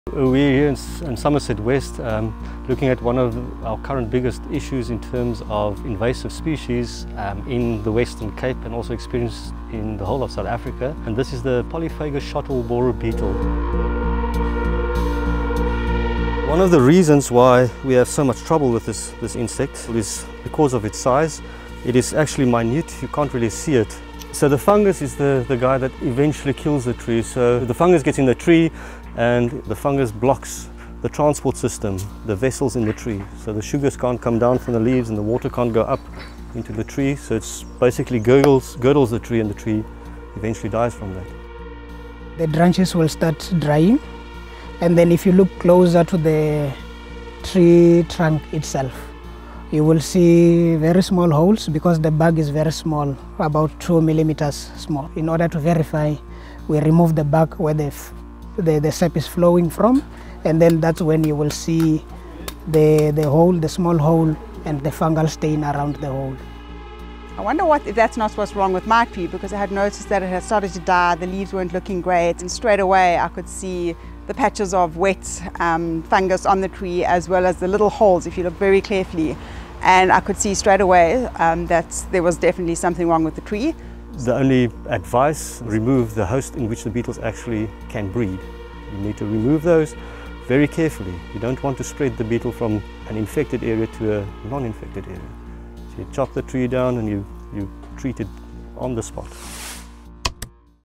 WATCH: An invasive beetle is killing SA’s trees - here’s how to spot the signs
Cape Town residents are being urged to be on the look-out after an invasive beetle was spotted in Somerset West. The Polyphagous shot hole borer (PSHB) beetle is native to Asia and has a symbiotic relationship with ambrosial fungi, which kills trees.

Here's how to spot the signs in your garden. Watch.